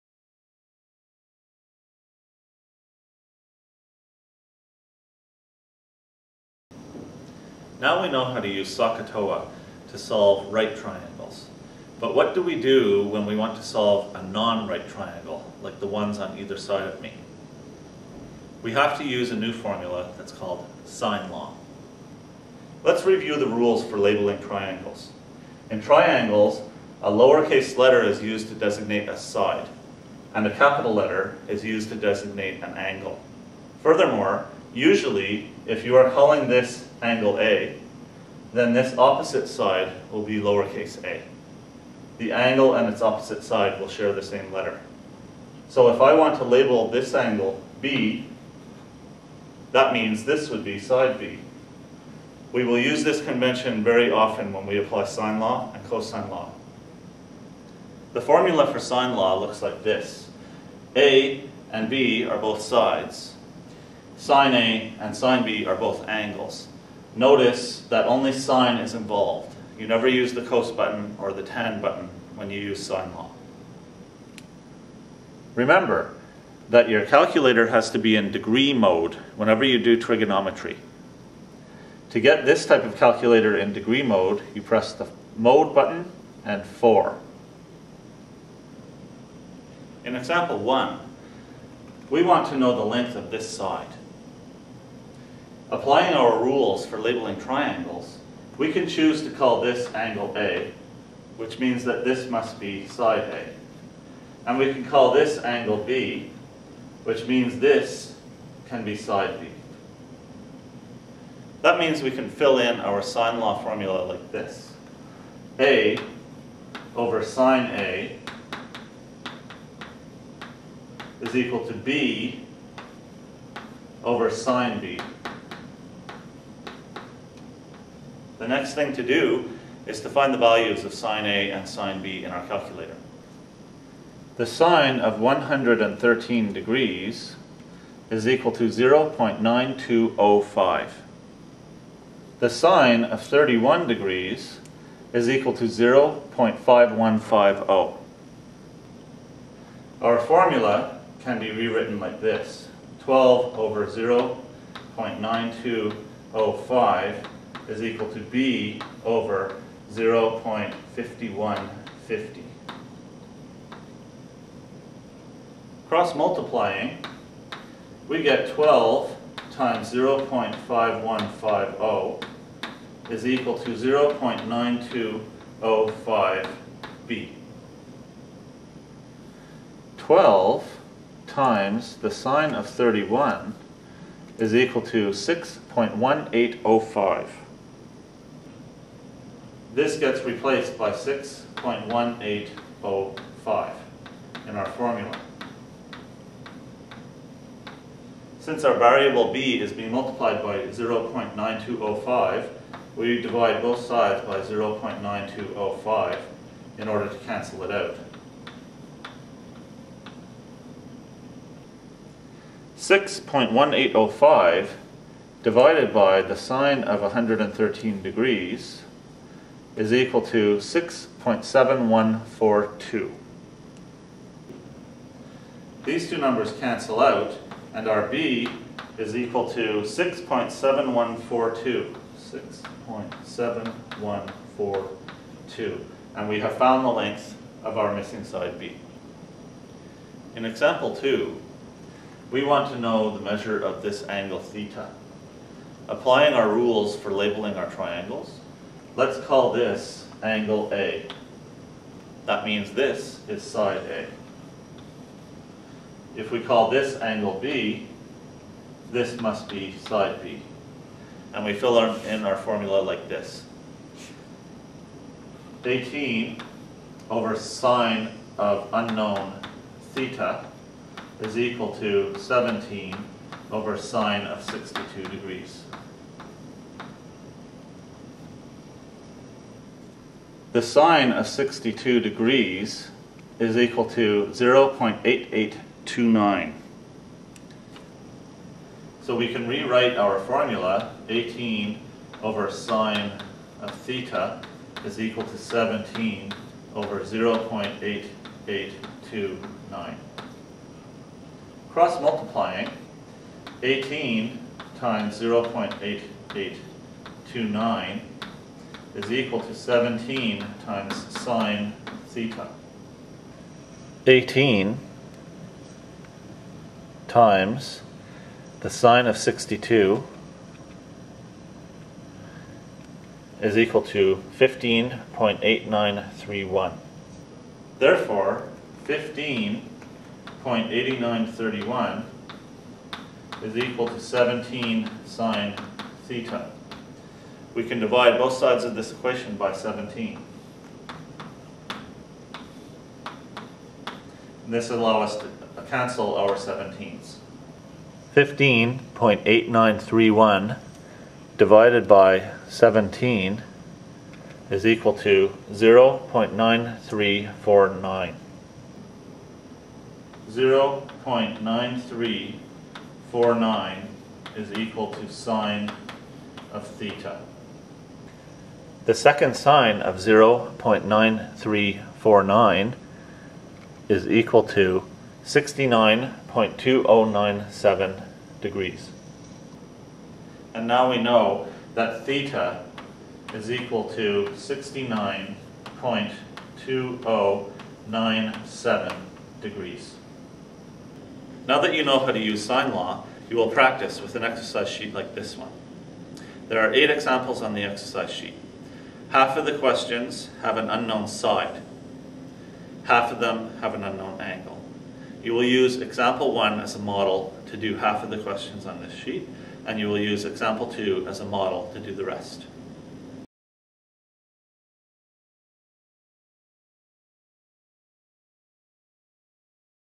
Using the Sine Law
This video illustrates how to use the Sine Law in high school and college trigonometry. For many more Math videos, as well as exercise and answer sheets, go to: